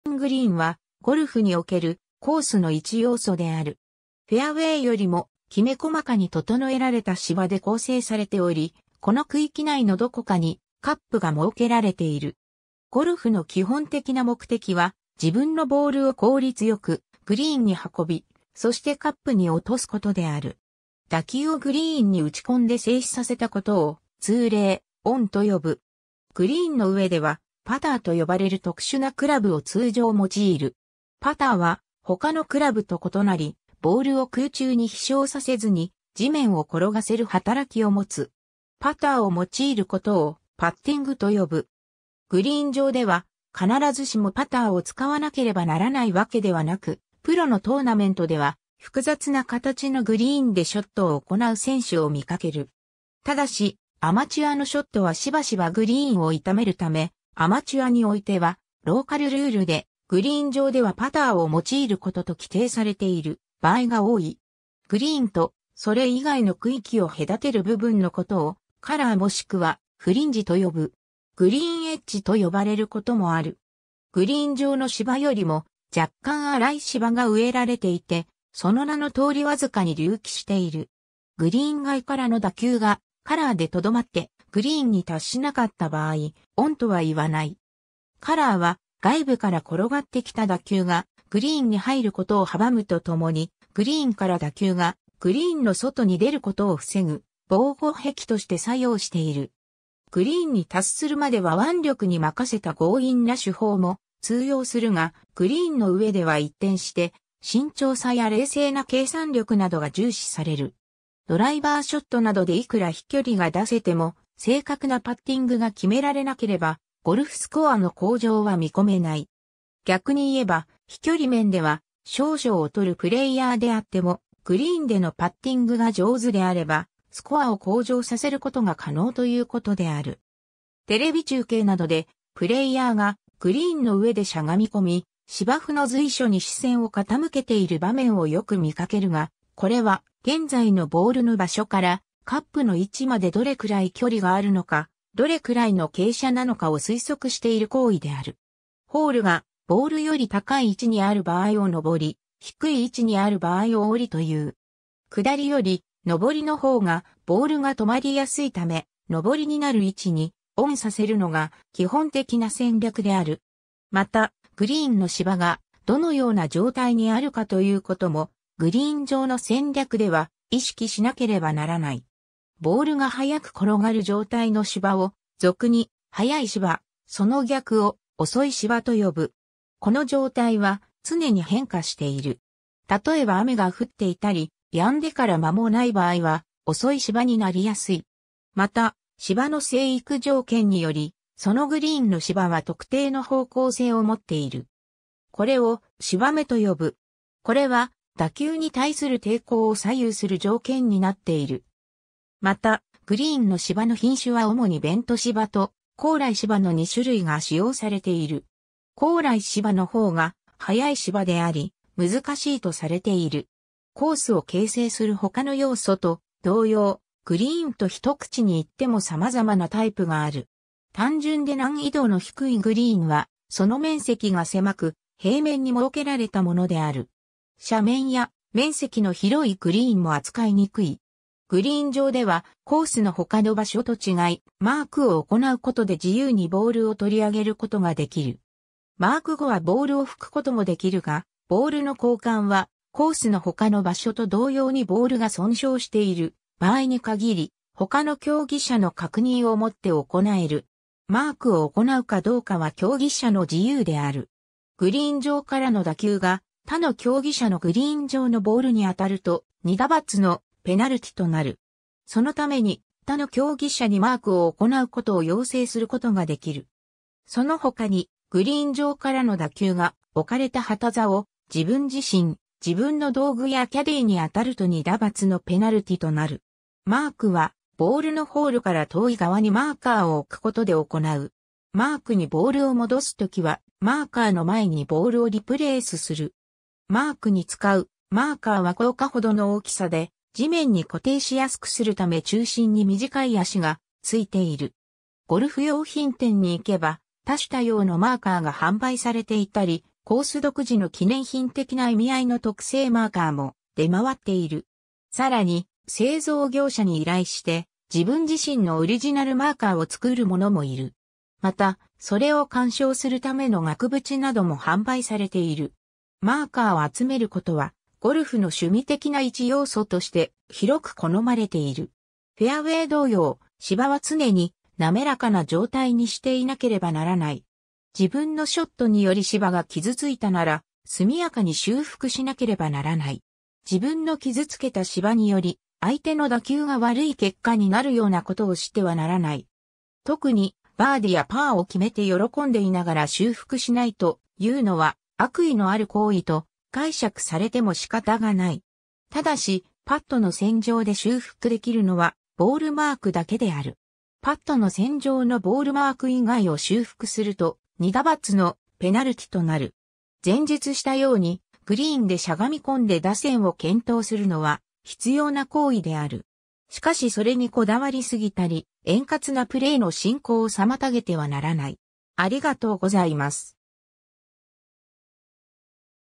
グリーン (ゴルフ)
グリーン グリーン（Green）は、ゴルフにおけるコースの一要素である。
フェアウェイよりもきめ細かに整えられた芝で構成されており、この区域内のどこかにカップ（ホールとも呼ばれる）が設けられている。
ゴルフの基本的な目的は、自分のボールを効率よくグリーンに運び、そしてカップに落とすことである。
 打球をグリーンに打ち込んで静止させたことを、通例「オン」と呼ぶ。
 グリーンの上では、パターと呼ばれる特殊なクラブを通常用いる。
パターは他のクラブと異なり、ボールを空中に飛翔させずに、地面を転がせる働きを持つ。
パターを用いることを「パッティング」と呼ぶ。
グリーン上では、必ずしもパターを使わなければならないわけではなく、プロのトーナメントでは複雑な形のグリーンでショットを行う選手を見かける。
ただし、アマチュアのショットはしばしばグリーンを傷めるため、アマチュアにおいてはローカルルールで「グリーン上ではパターを用いること」と規定されている場合が多い。
 グリーンと、それ以外の区域を隔てる部分のことを「カラー」（Collar、襟の意味）もしくは「フリンジ」（Fringe、縁の意味）と呼ぶ。
「グリーンエッジ」（Green edge、グリーンの縁の意味）と呼ばれることもある。
グリーン上の芝よりも若干荒い芝が植えられていて、その名の通りわずかに隆起している。
グリーン外からの打球がカラーでとどまってグリーンに達しなかった場合、「オン」とはいわない。
カラーは外部から転がってきた打球がグリーンに入ることを阻むとともに、グリーンから(誤って強く打ってしまった)打球がグリーンの外に出ることを防ぐ防護壁として作用している。
 グリーンに達するまでは腕力に任せた強引な手法も通用するが、グリーンの上では一転して慎重さや冷静な計算力などが重視される。
ドライバーショットなどでいくら飛距離が出せても、正確なパッティングが決められなければ、ゴルフスコアの向上は見込めない。
逆に言えば、飛距離面では少々劣るプレイヤーであっても、グリーンでのパッティングが上手であれば、スコアを向上させることが可能という事である。
 テレビ中継などで、プレイヤーがグリーンの上でしゃがみ込み芝生の随所に視線を傾けている場面をよく見かけるが、これは現在のボールの場所からカップの位置までどれくらい距離があるのか、どれくらいの傾斜なのかを推測している行為である。
 ホールがボールより高い位置にある場合を上り、低い位置にある場合を下りと言う。
下りより上りのほうがボールが止まりやすいため、上りになる位置に「オン」させるのが基本的な戦略である。
また、グリーンの芝がどのような状態にあるかということも、グリーン上の戦略では意識しなければならない。
ボールが早く転がる状態の芝を、俗に「速い芝」・その逆を「遅い芝」と呼ぶ。
この状態は、常に変化している。
たとえば雨が降っていたり、やんでから間もない場合は「遅い芝」になりやすい。
また、芝の生育条件により、そのグリーンの芝は特定の方向性をもっている。
これを「芝目」と呼ぶ。
これは、打球に対する抵抗を左右する条件になっている。
また、グリーンの芝の品種は主にベント芝と高麗芝の2種類が使用されている。
高麗芝のほうが「速い芝」であり、難しいとされている。
 コースを形成するほかの要素と同様、グリーンと一口に言っても様々なタイプがある。
単純で難易度の低いグリーンは、その面積が狭く、平面に設けられたものである。
斜面や、面積の広いグリーンも扱いにくい。
 グリーン上ではコースの他の場所と違い、マークを行うことで自由にボールを取り上げることができる。
マーク後はボールを拭くこともできるが、ボールの交換はコースのほかの場所と同様にボールが損傷している場合に限り、他の競技者の確認を持って行える。
マークを行うかどうかは競技者の自由である。
グリーン上からの打球が、他の競技者のグリーン上のボールにあたると、2打罰のペナルティとなる。
そのために、他の競技者にマークを行うことを要請することができる。
そのほかにグリーン上からの打球が、置かれた旗竿、自分自身、自分の道具やキャディーにあたると2打罰のペナルティとなる。
 マークはボールのホールから遠い側にマーカーを置くことで行う。
マークにボールを戻すときは、マーカーの前にボールをリプレースする。
マークに使う「マーカー」は硬貨（1円玉）程の大きさで、地面に固定しやすくするため中心に短い足がついている。
ゴルフ用品店に行けば多種多...